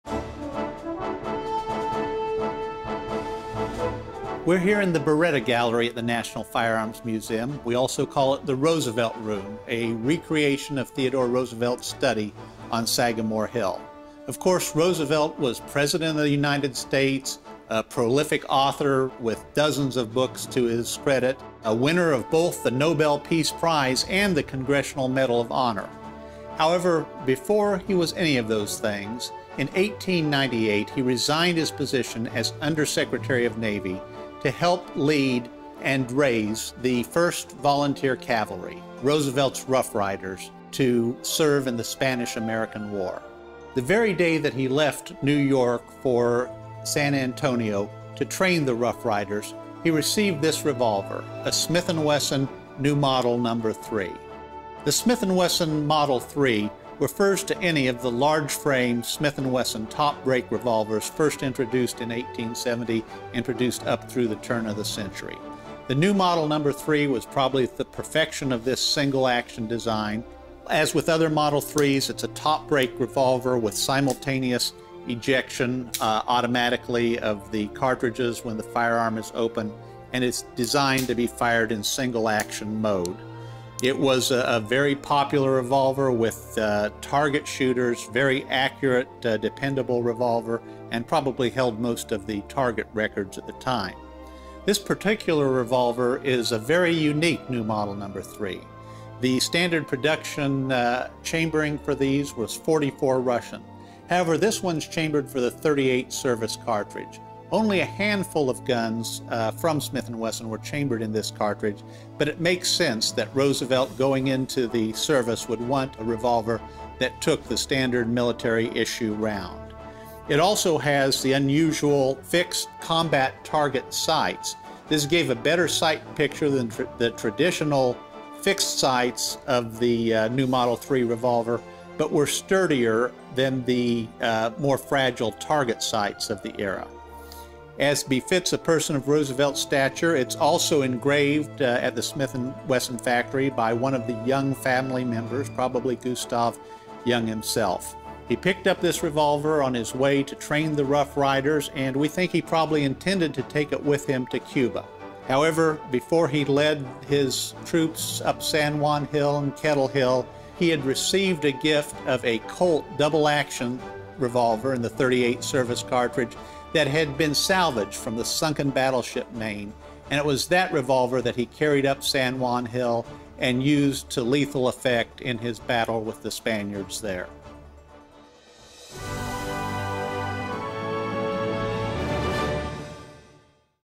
NFM Treasure Gun - Theodore Roosevelt's S&W New Model No. 3 Revolver
Engraved Smith and Wesson New Model Number Three single action revolver made expressly for Theodore Roosevelt, at the time formed the Rough Riders.  A Treasure Gun from the NRA National Firearms Museum.  Narrated by Jim Supica